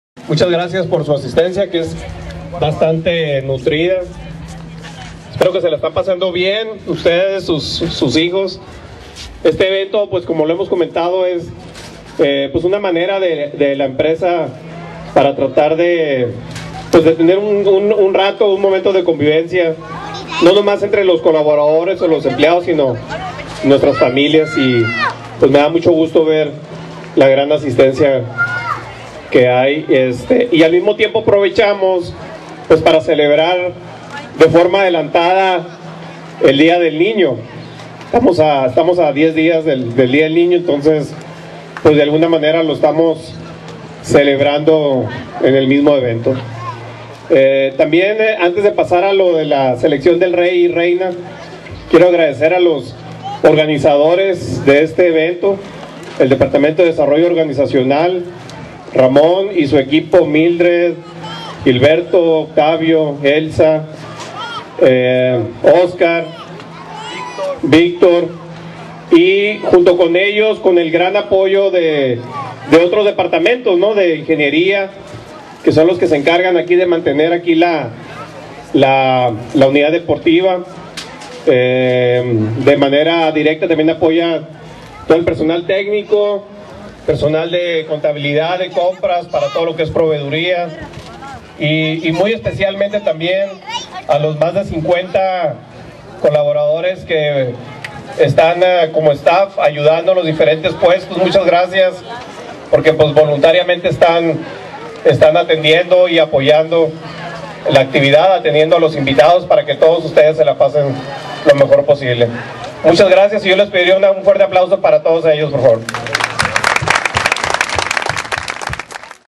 IV CONVIVENCIA FAMILIAR MTK
La empresa Maquilas Teta Kawi, realizó su edición numero IV, de la Gran Convivencia Familiar, que se hace al calor del día del niño, ofreciendo a sus empleados e hijos de empleados un día de sana diversión en la que les ofrecieron dulces, regalos, juegos y muchas diversión.